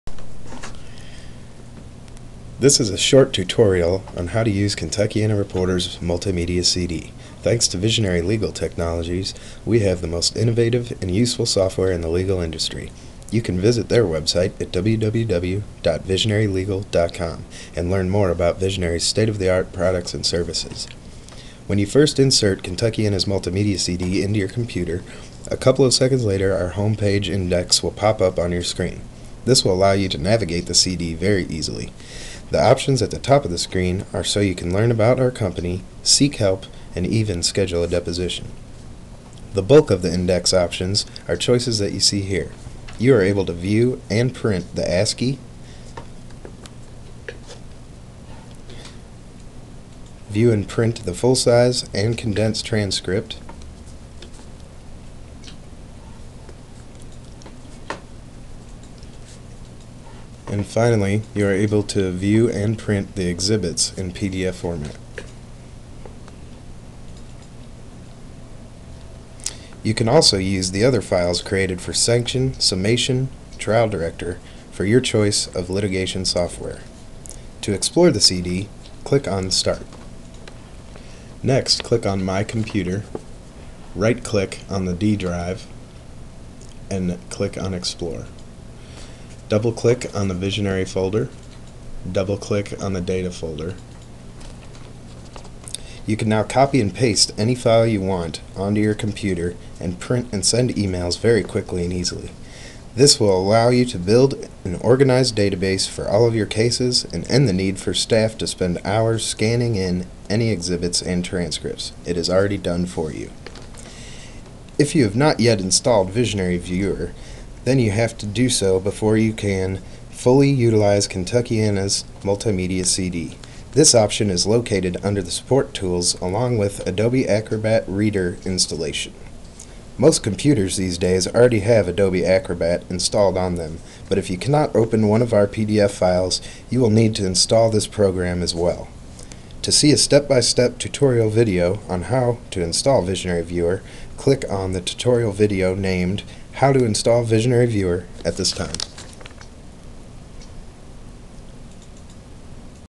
Kentuckiana Reporters Multimedia CD Rom
Instructions on navigating through Kentuckiana Reporters' multimedia CD Rom.